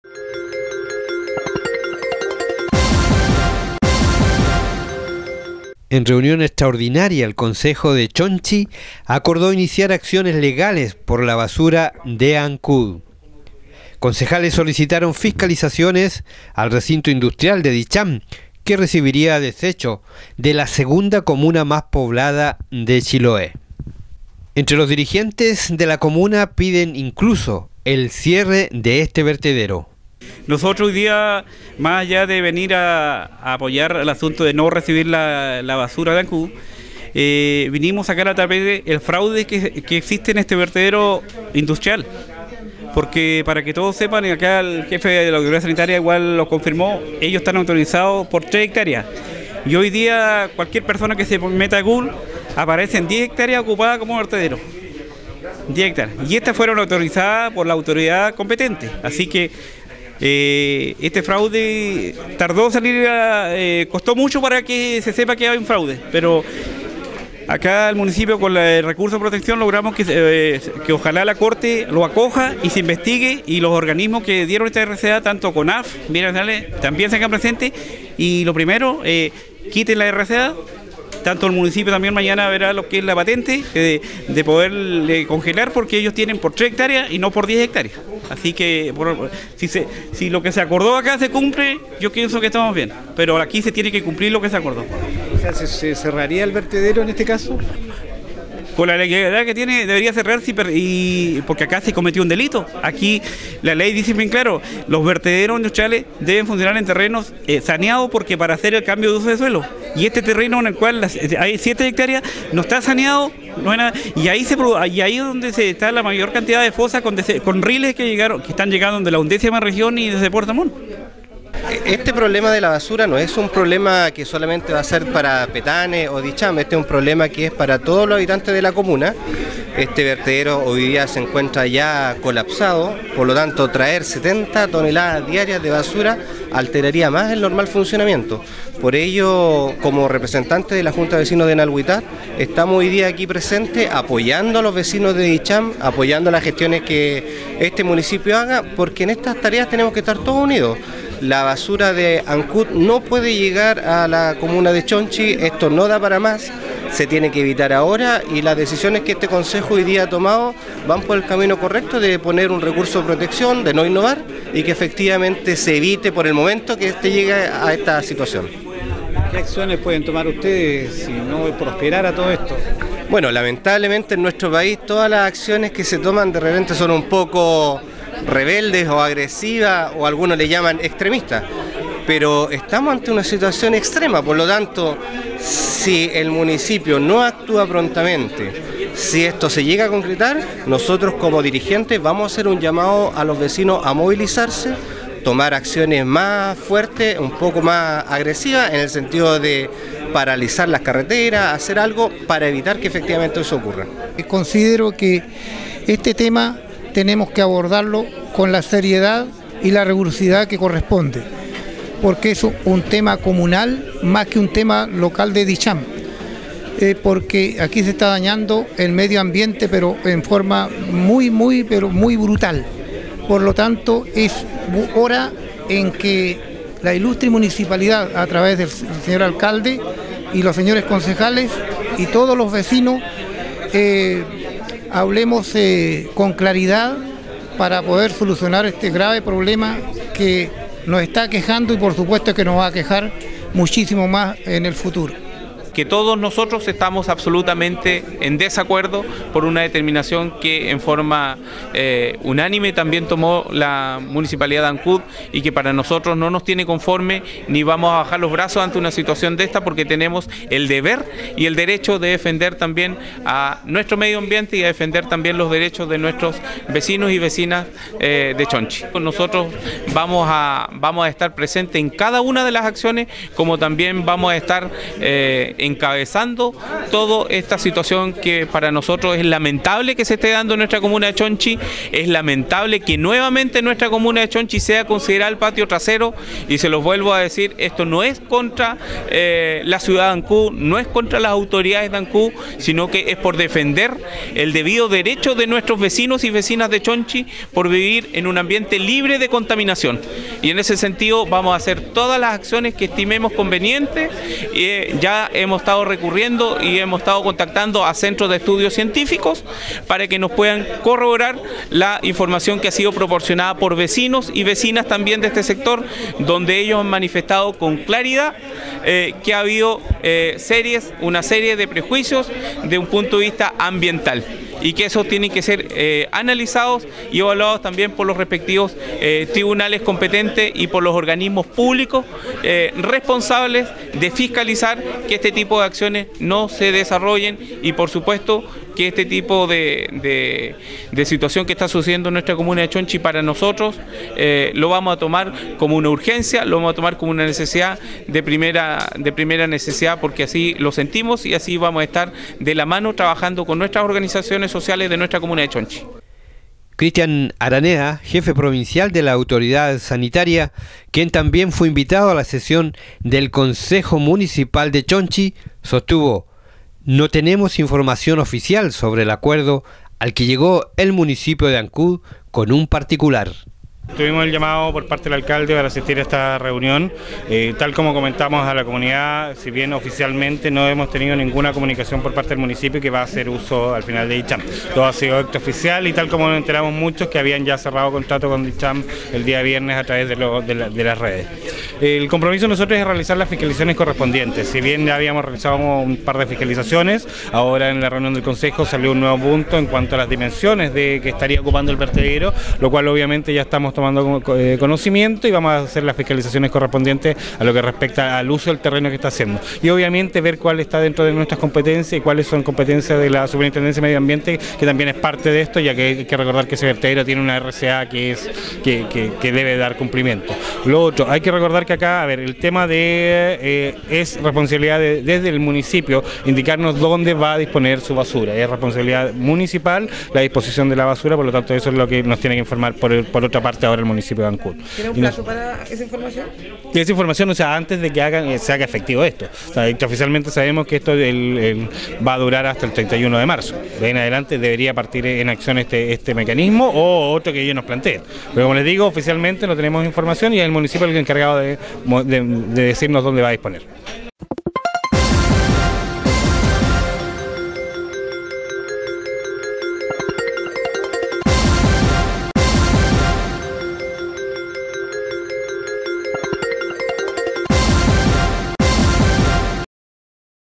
RESUMEN SEMANAL VERTEDERO CHONCHI
REUNIÓN SOSTENIDA EN LA COMUNA DE CHONCHI EN RELACIÓN AL VERTEDERO Y ACCIONES A SEGUIR DURANTE LA SEMANA